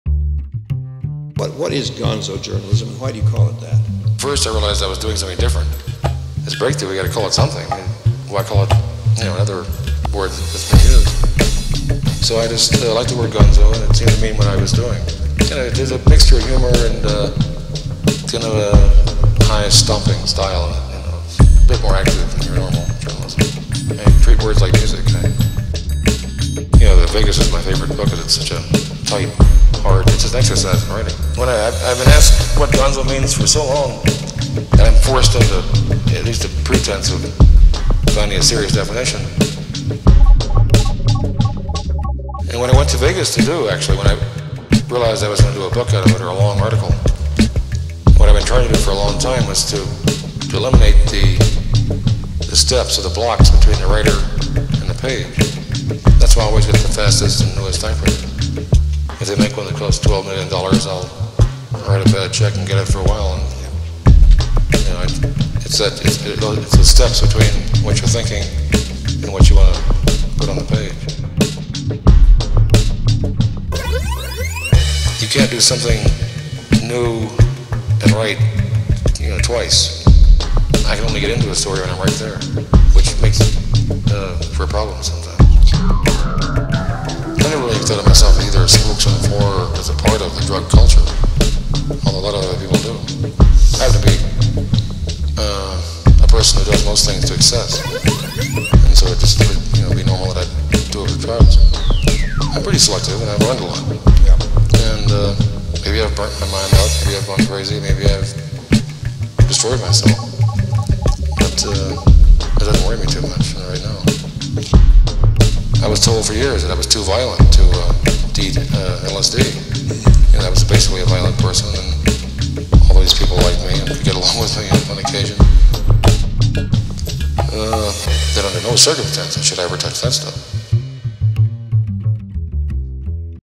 What is Gonzo Journalism?
In "What is Gonzo Journalism," I explore the wild, unfiltered world of Hunter S. Thompson, the maverick journalist who revolutionized the field with his raw, immersive style. Thompson's approach, known as Gonzo Journalism, blurred the lines between observer and participant, making the writer a central character in the narrative. This piece delves into Thompson's own reflections on Gonzo, capturing his unique perspective on titles, drugs, and the various isms that shaped his worldview.

Thompson’s voice guides this portrait, offering insights into his chaotic, yet profoundly insightful method of storytelling. His thoughts on the power of titles and the influence of substances on his work are woven into the narrative, creating a visceral and authentic representation of the man behind the myth. Through this piece, I aim to capture the essence of Thompson’s fearless approach to journalism, where the pursuit of truth was inseparable from the pursuit of experience.

This work is not just a tribute to Thompson’s legacy, but also a commentary on the nature of truth, identity, and the creative process. By immersing the viewer in Thompson’s world, I invite them to question the boundaries of reality and fiction, much as Thompson did throughout his career. "What is Gonzo Journalism" is a celebration of a style that continues to influence writers and artists alike, pushing the limits of what it means to tell a story.